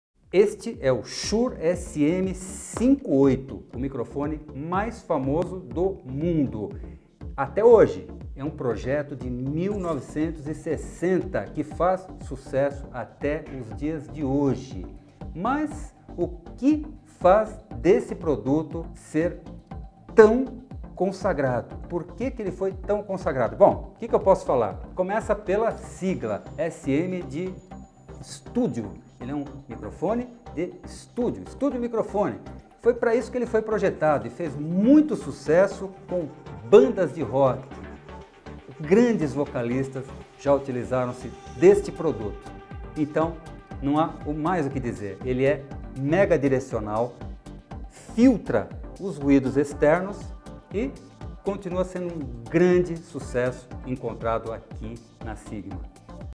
Microfone vocal - Shure SM58 LC
O microfone vocal Shure SM58-LC apresenta um design vocal dinâmico unidirecional (cardióide), adequado para uso vocal profissional em apresentações ao vivo, reforço de som e gravação em estúdio.